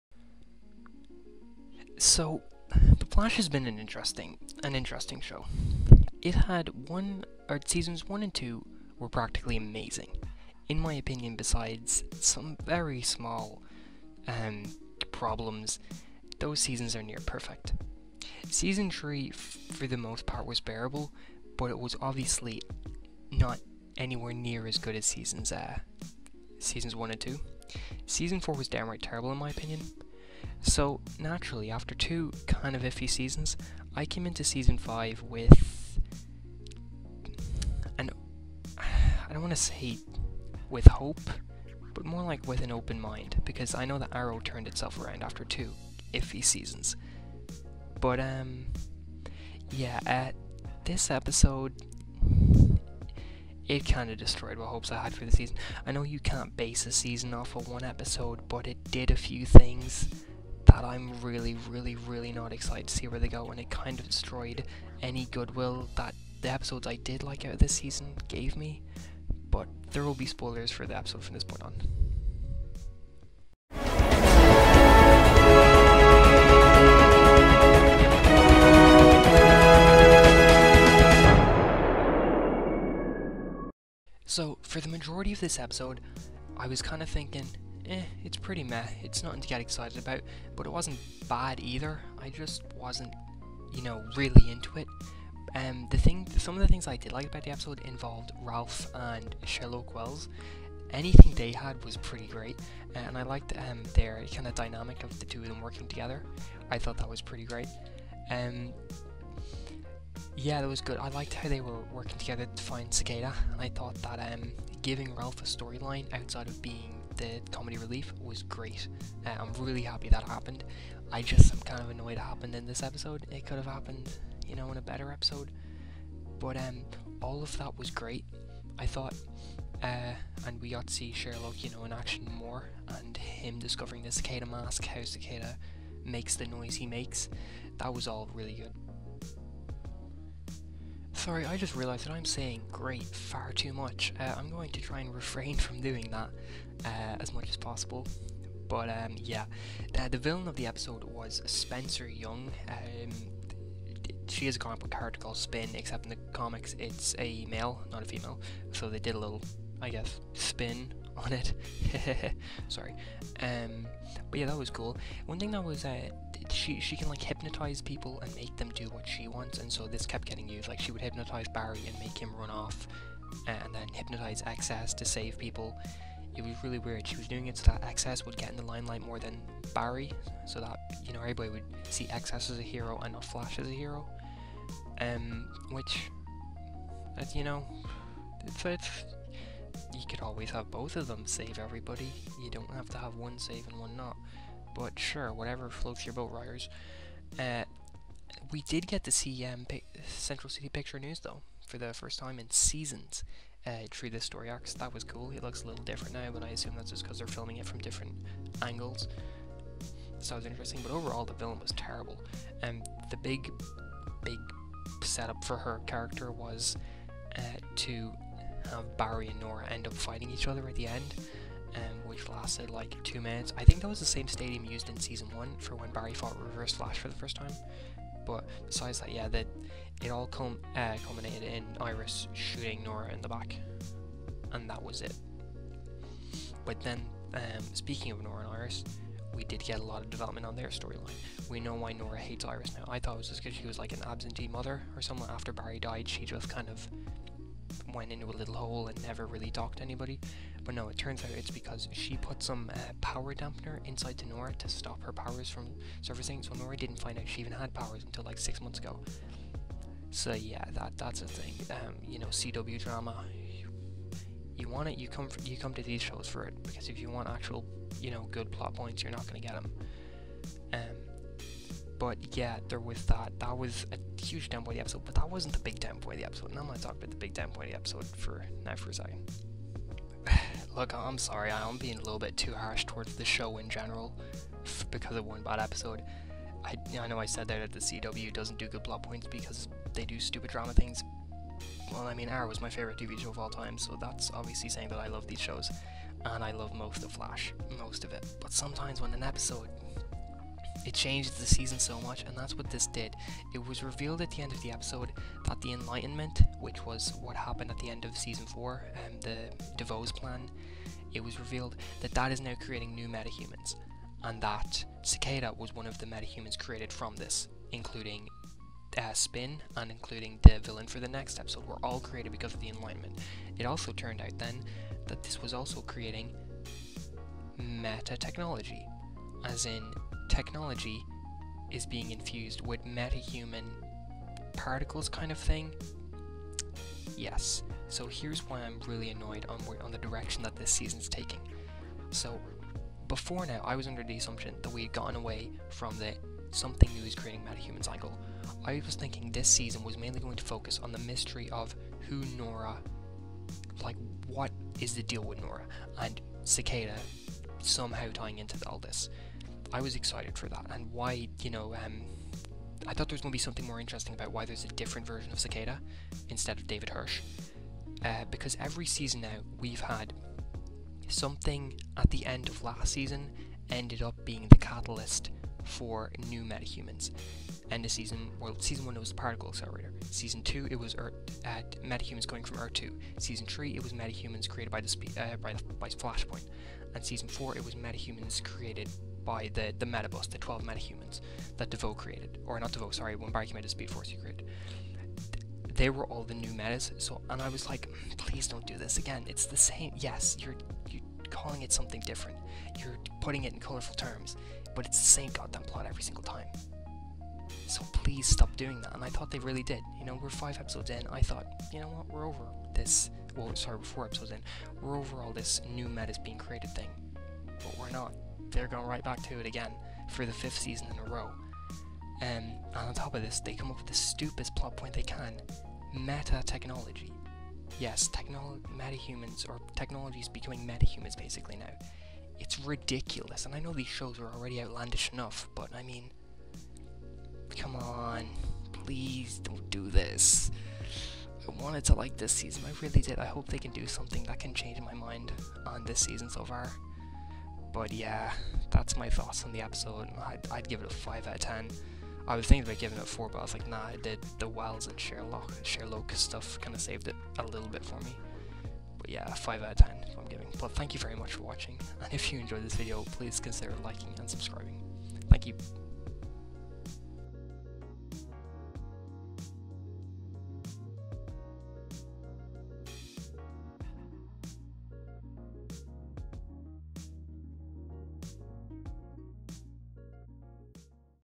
WORST Episode of the Season Yet? - The Flash 5x04 'News Flash' Review
My review for The Flash 5x04.